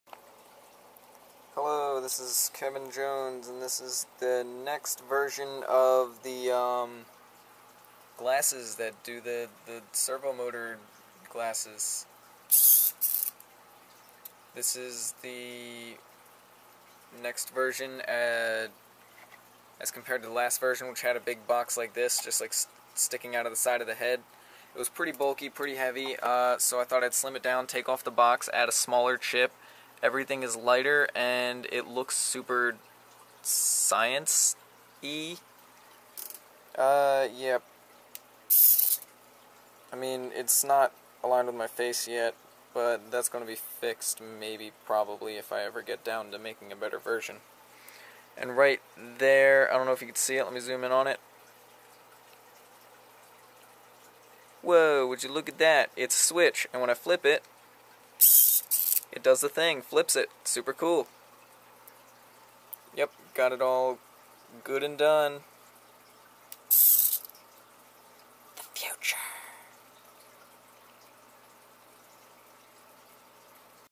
arduino servo motor glasses MK2
a quick update on the glasses that look flippin amazing